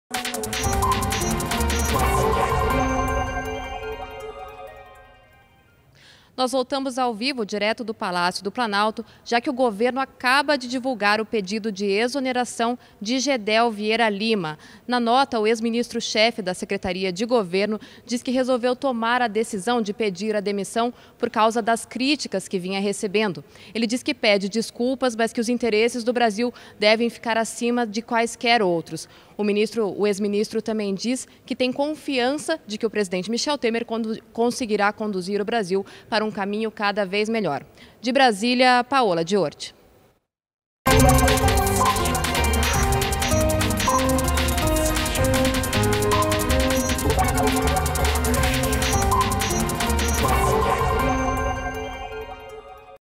Geddel Vieira Lima é exonerado da Secretaria de Governo
REPÓRTER NBR - 25.11.16: Geddel Vieira Lima, ex-ministro da Secretaria de Governo, foi exonerado do cargo a pedido nesta sexta-feira (25). Em nota, Geddel pediu desculpas e afirmou que os interesses do Brasil devem ficar acima de quaisquer outros.